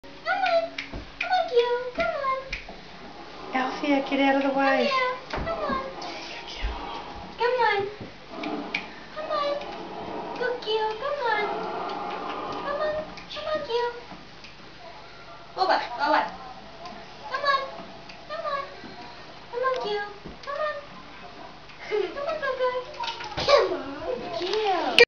Coco Finally Learns How To Use The Cat Door!
If I hadn't seen it myself I wouldn't have believed it.